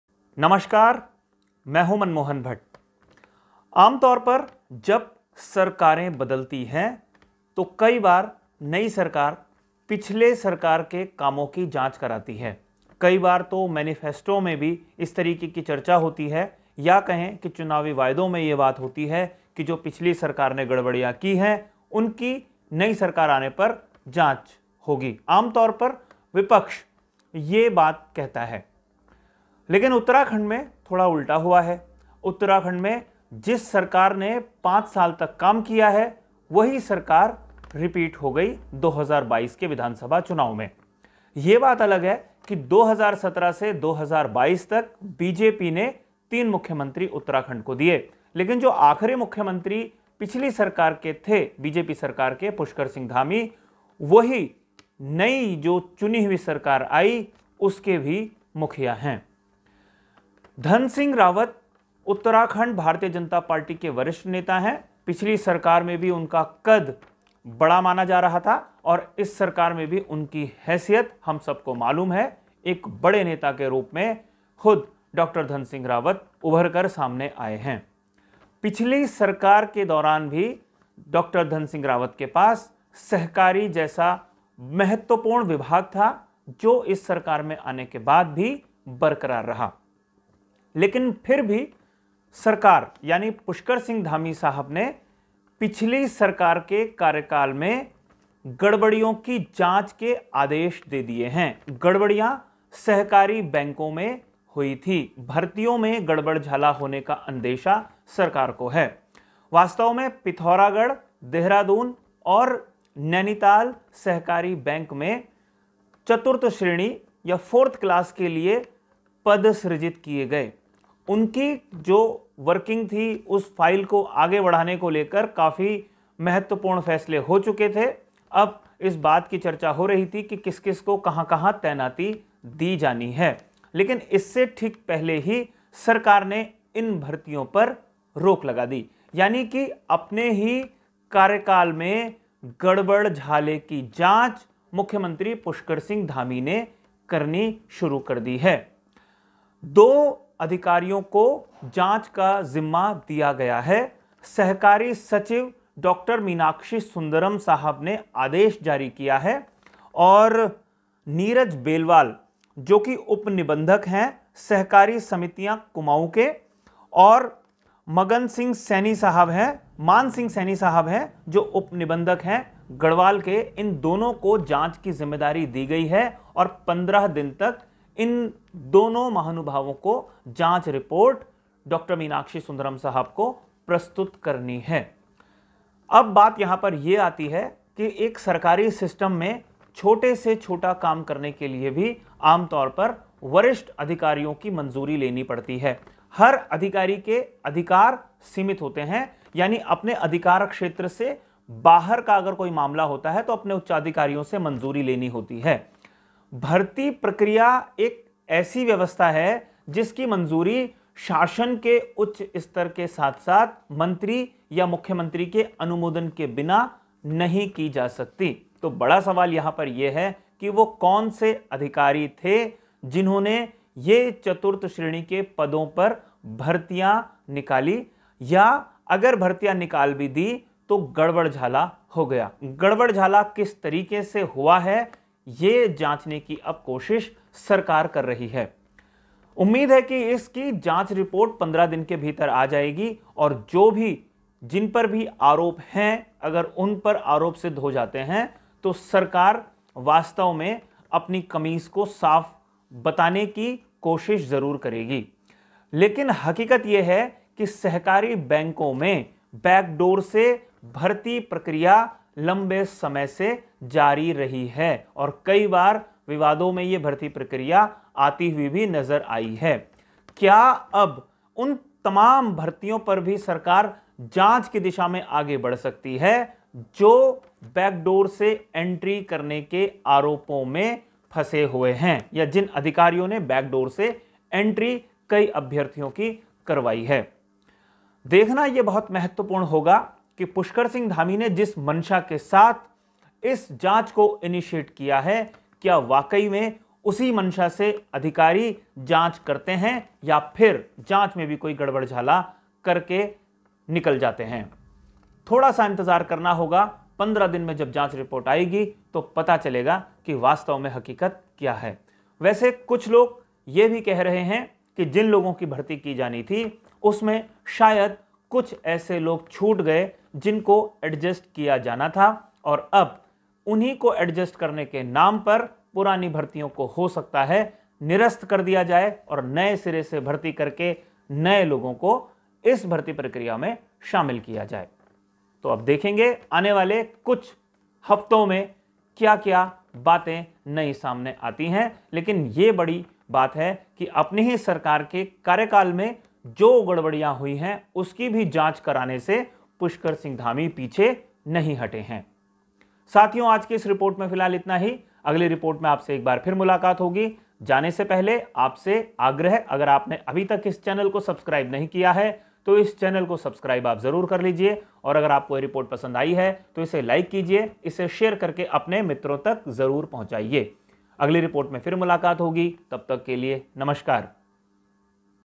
अपनी ही सरकार में हुई गड़बड़ी की जांच कराने से भी नहीं चुके CM पुष्कर सिंह धामी.Manmohan_Bhatt
उत्तराखंड के मुख्यमंत्री पुष्कर सिंह धामी ने अपनी ही सरकार में हुए गड़बड़ झाले की जांच के आदेश दिए हैं। यह जांच सहकारिता विभाग में चतुर्थ श्रेणी के कर्मचारियों की भर्ती के मामले में शुरू की गई है । सहकारिता विभाग के पिथौरागढ़ नैनीताल और देहरादून बैंक में चतुर्थ श्रेणी के पदों को सृजित कर भर्ती प्रक्रिया शुरू की गई थी।

लेकिन इस भर्ती प्रक्रिया में अनियमितता सामने आने के बाद मुख्यमंत्री पुष्कर सिंह धामी ने जांच के आदेश दिए हैं। उत्तराखंड के सहकारिता विभाग के सचिव डॉ मीनाक्षी सुंदरम ने जांच के आदेश दिए हैं। गढ़वाल और कुमाऊं मंडल में उप निबंधक सहकारिता को जांच की जिम्मेदारी दी गई है। यह जांच रिपोर्ट 15 दिन में ही प्रस्तुत करनी होगी।

 वैसे उत्तराखंड में सहकारिता विभाग के बैंक्स में बैक डोर से भर्ती प्रक्रिया विवाद लगातार बनाते रहते हैं। कई बार इसको लेकर पहले भी गड़बड़झाला सामने आया है। अब सवाल यह भी उठ रहा है कि क्या दूसरी भर्ती प्रक्रिया में भी इसी तरीके की अनियमितता सामने आई है। क्या इसकी भी भविष्य में जांच की जा सकती है इस पर भी कई लोग सवाल पूछ रहे हैं।